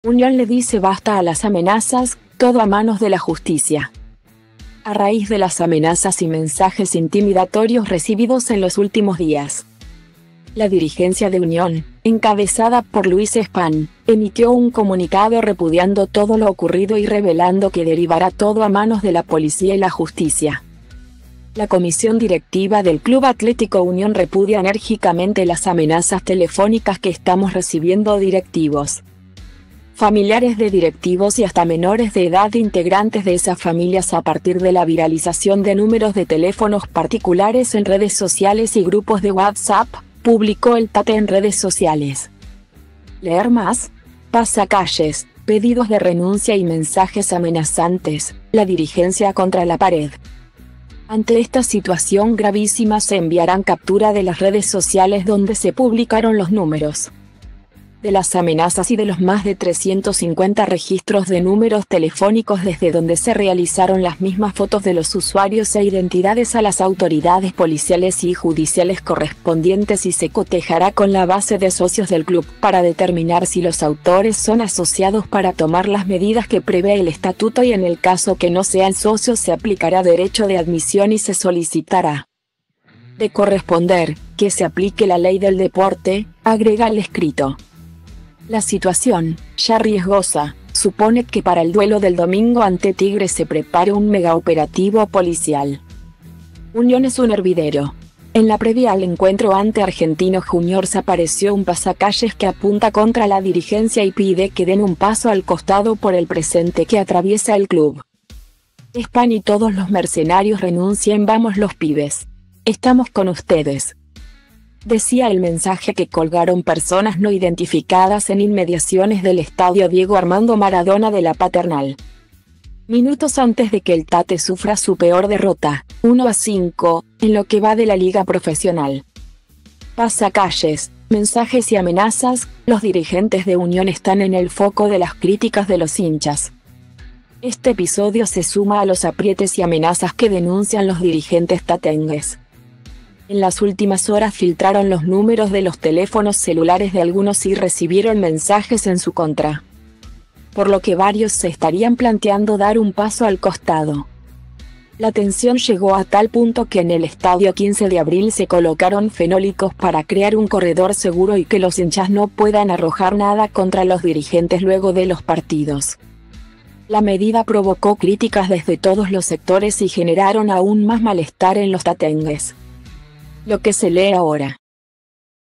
Unión le dice basta a las amenazas: todo a manos de la Justicia
A raíz de las amenazas y mensajes intimidatorios recibidos en los últimos días, la dirigencia de Unión, encabezada por Luis Spahn, emitió un comunicado repudiando todo lo ocurrido y revelando que derivará todo a manos de la Policía y la Justicia. "La Comisión Directiva del Club Atlético Unión repudia enérgicamente las amenazas telefónicas que estamos recibiendo directivos,